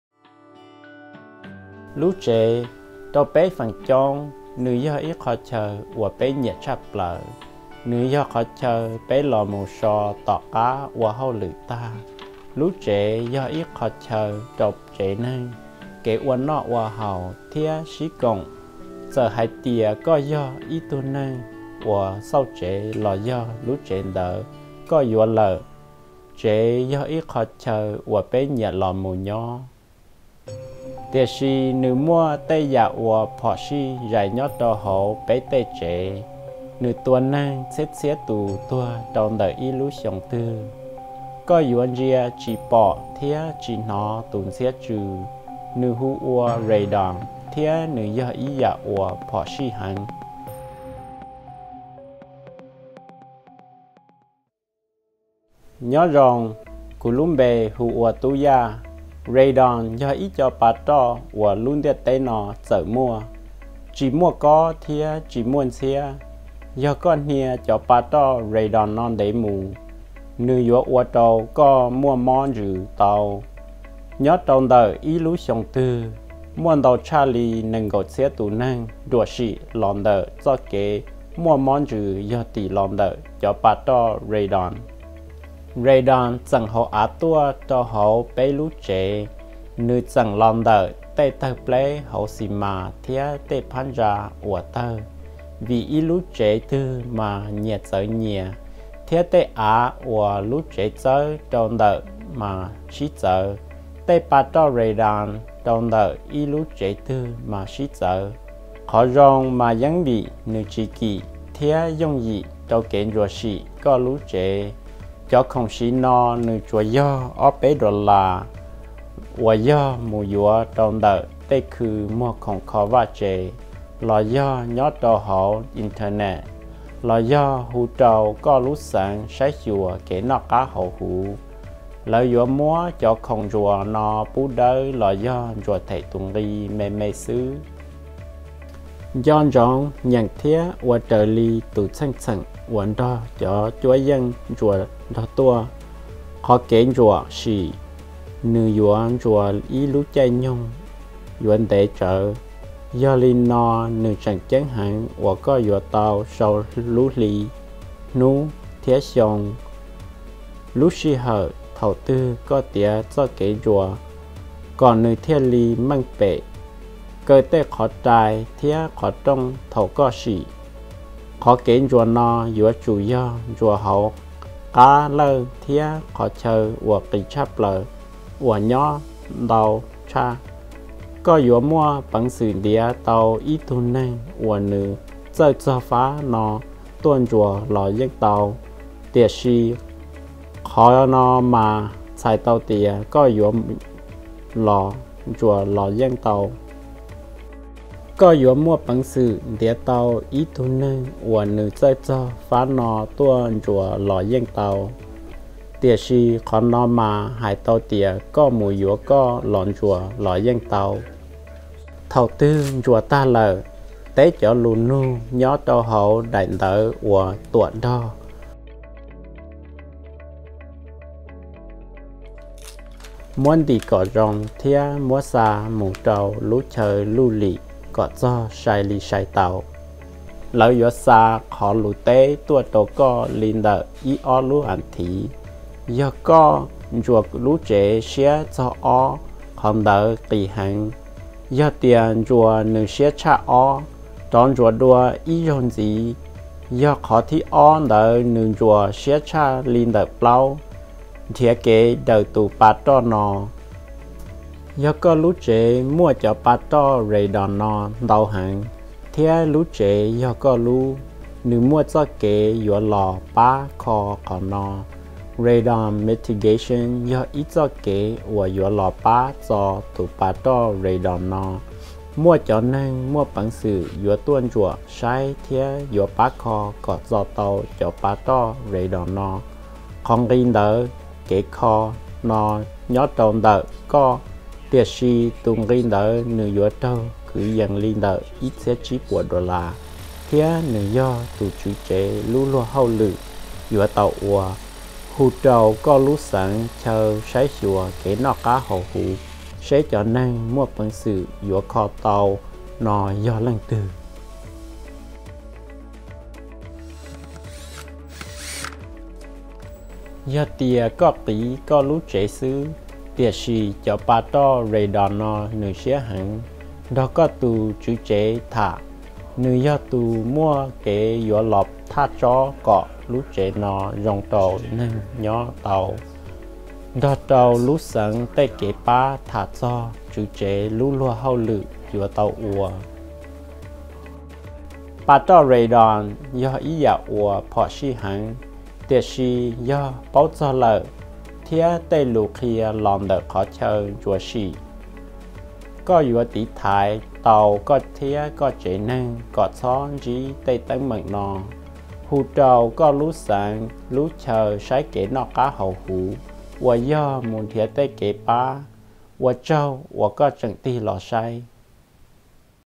Hmong - Radon Information - MDH Short
There is a danger hiding in many homes.  You cannot smell it or see it. Here is information about it in Hmong.
Prepared by ECHO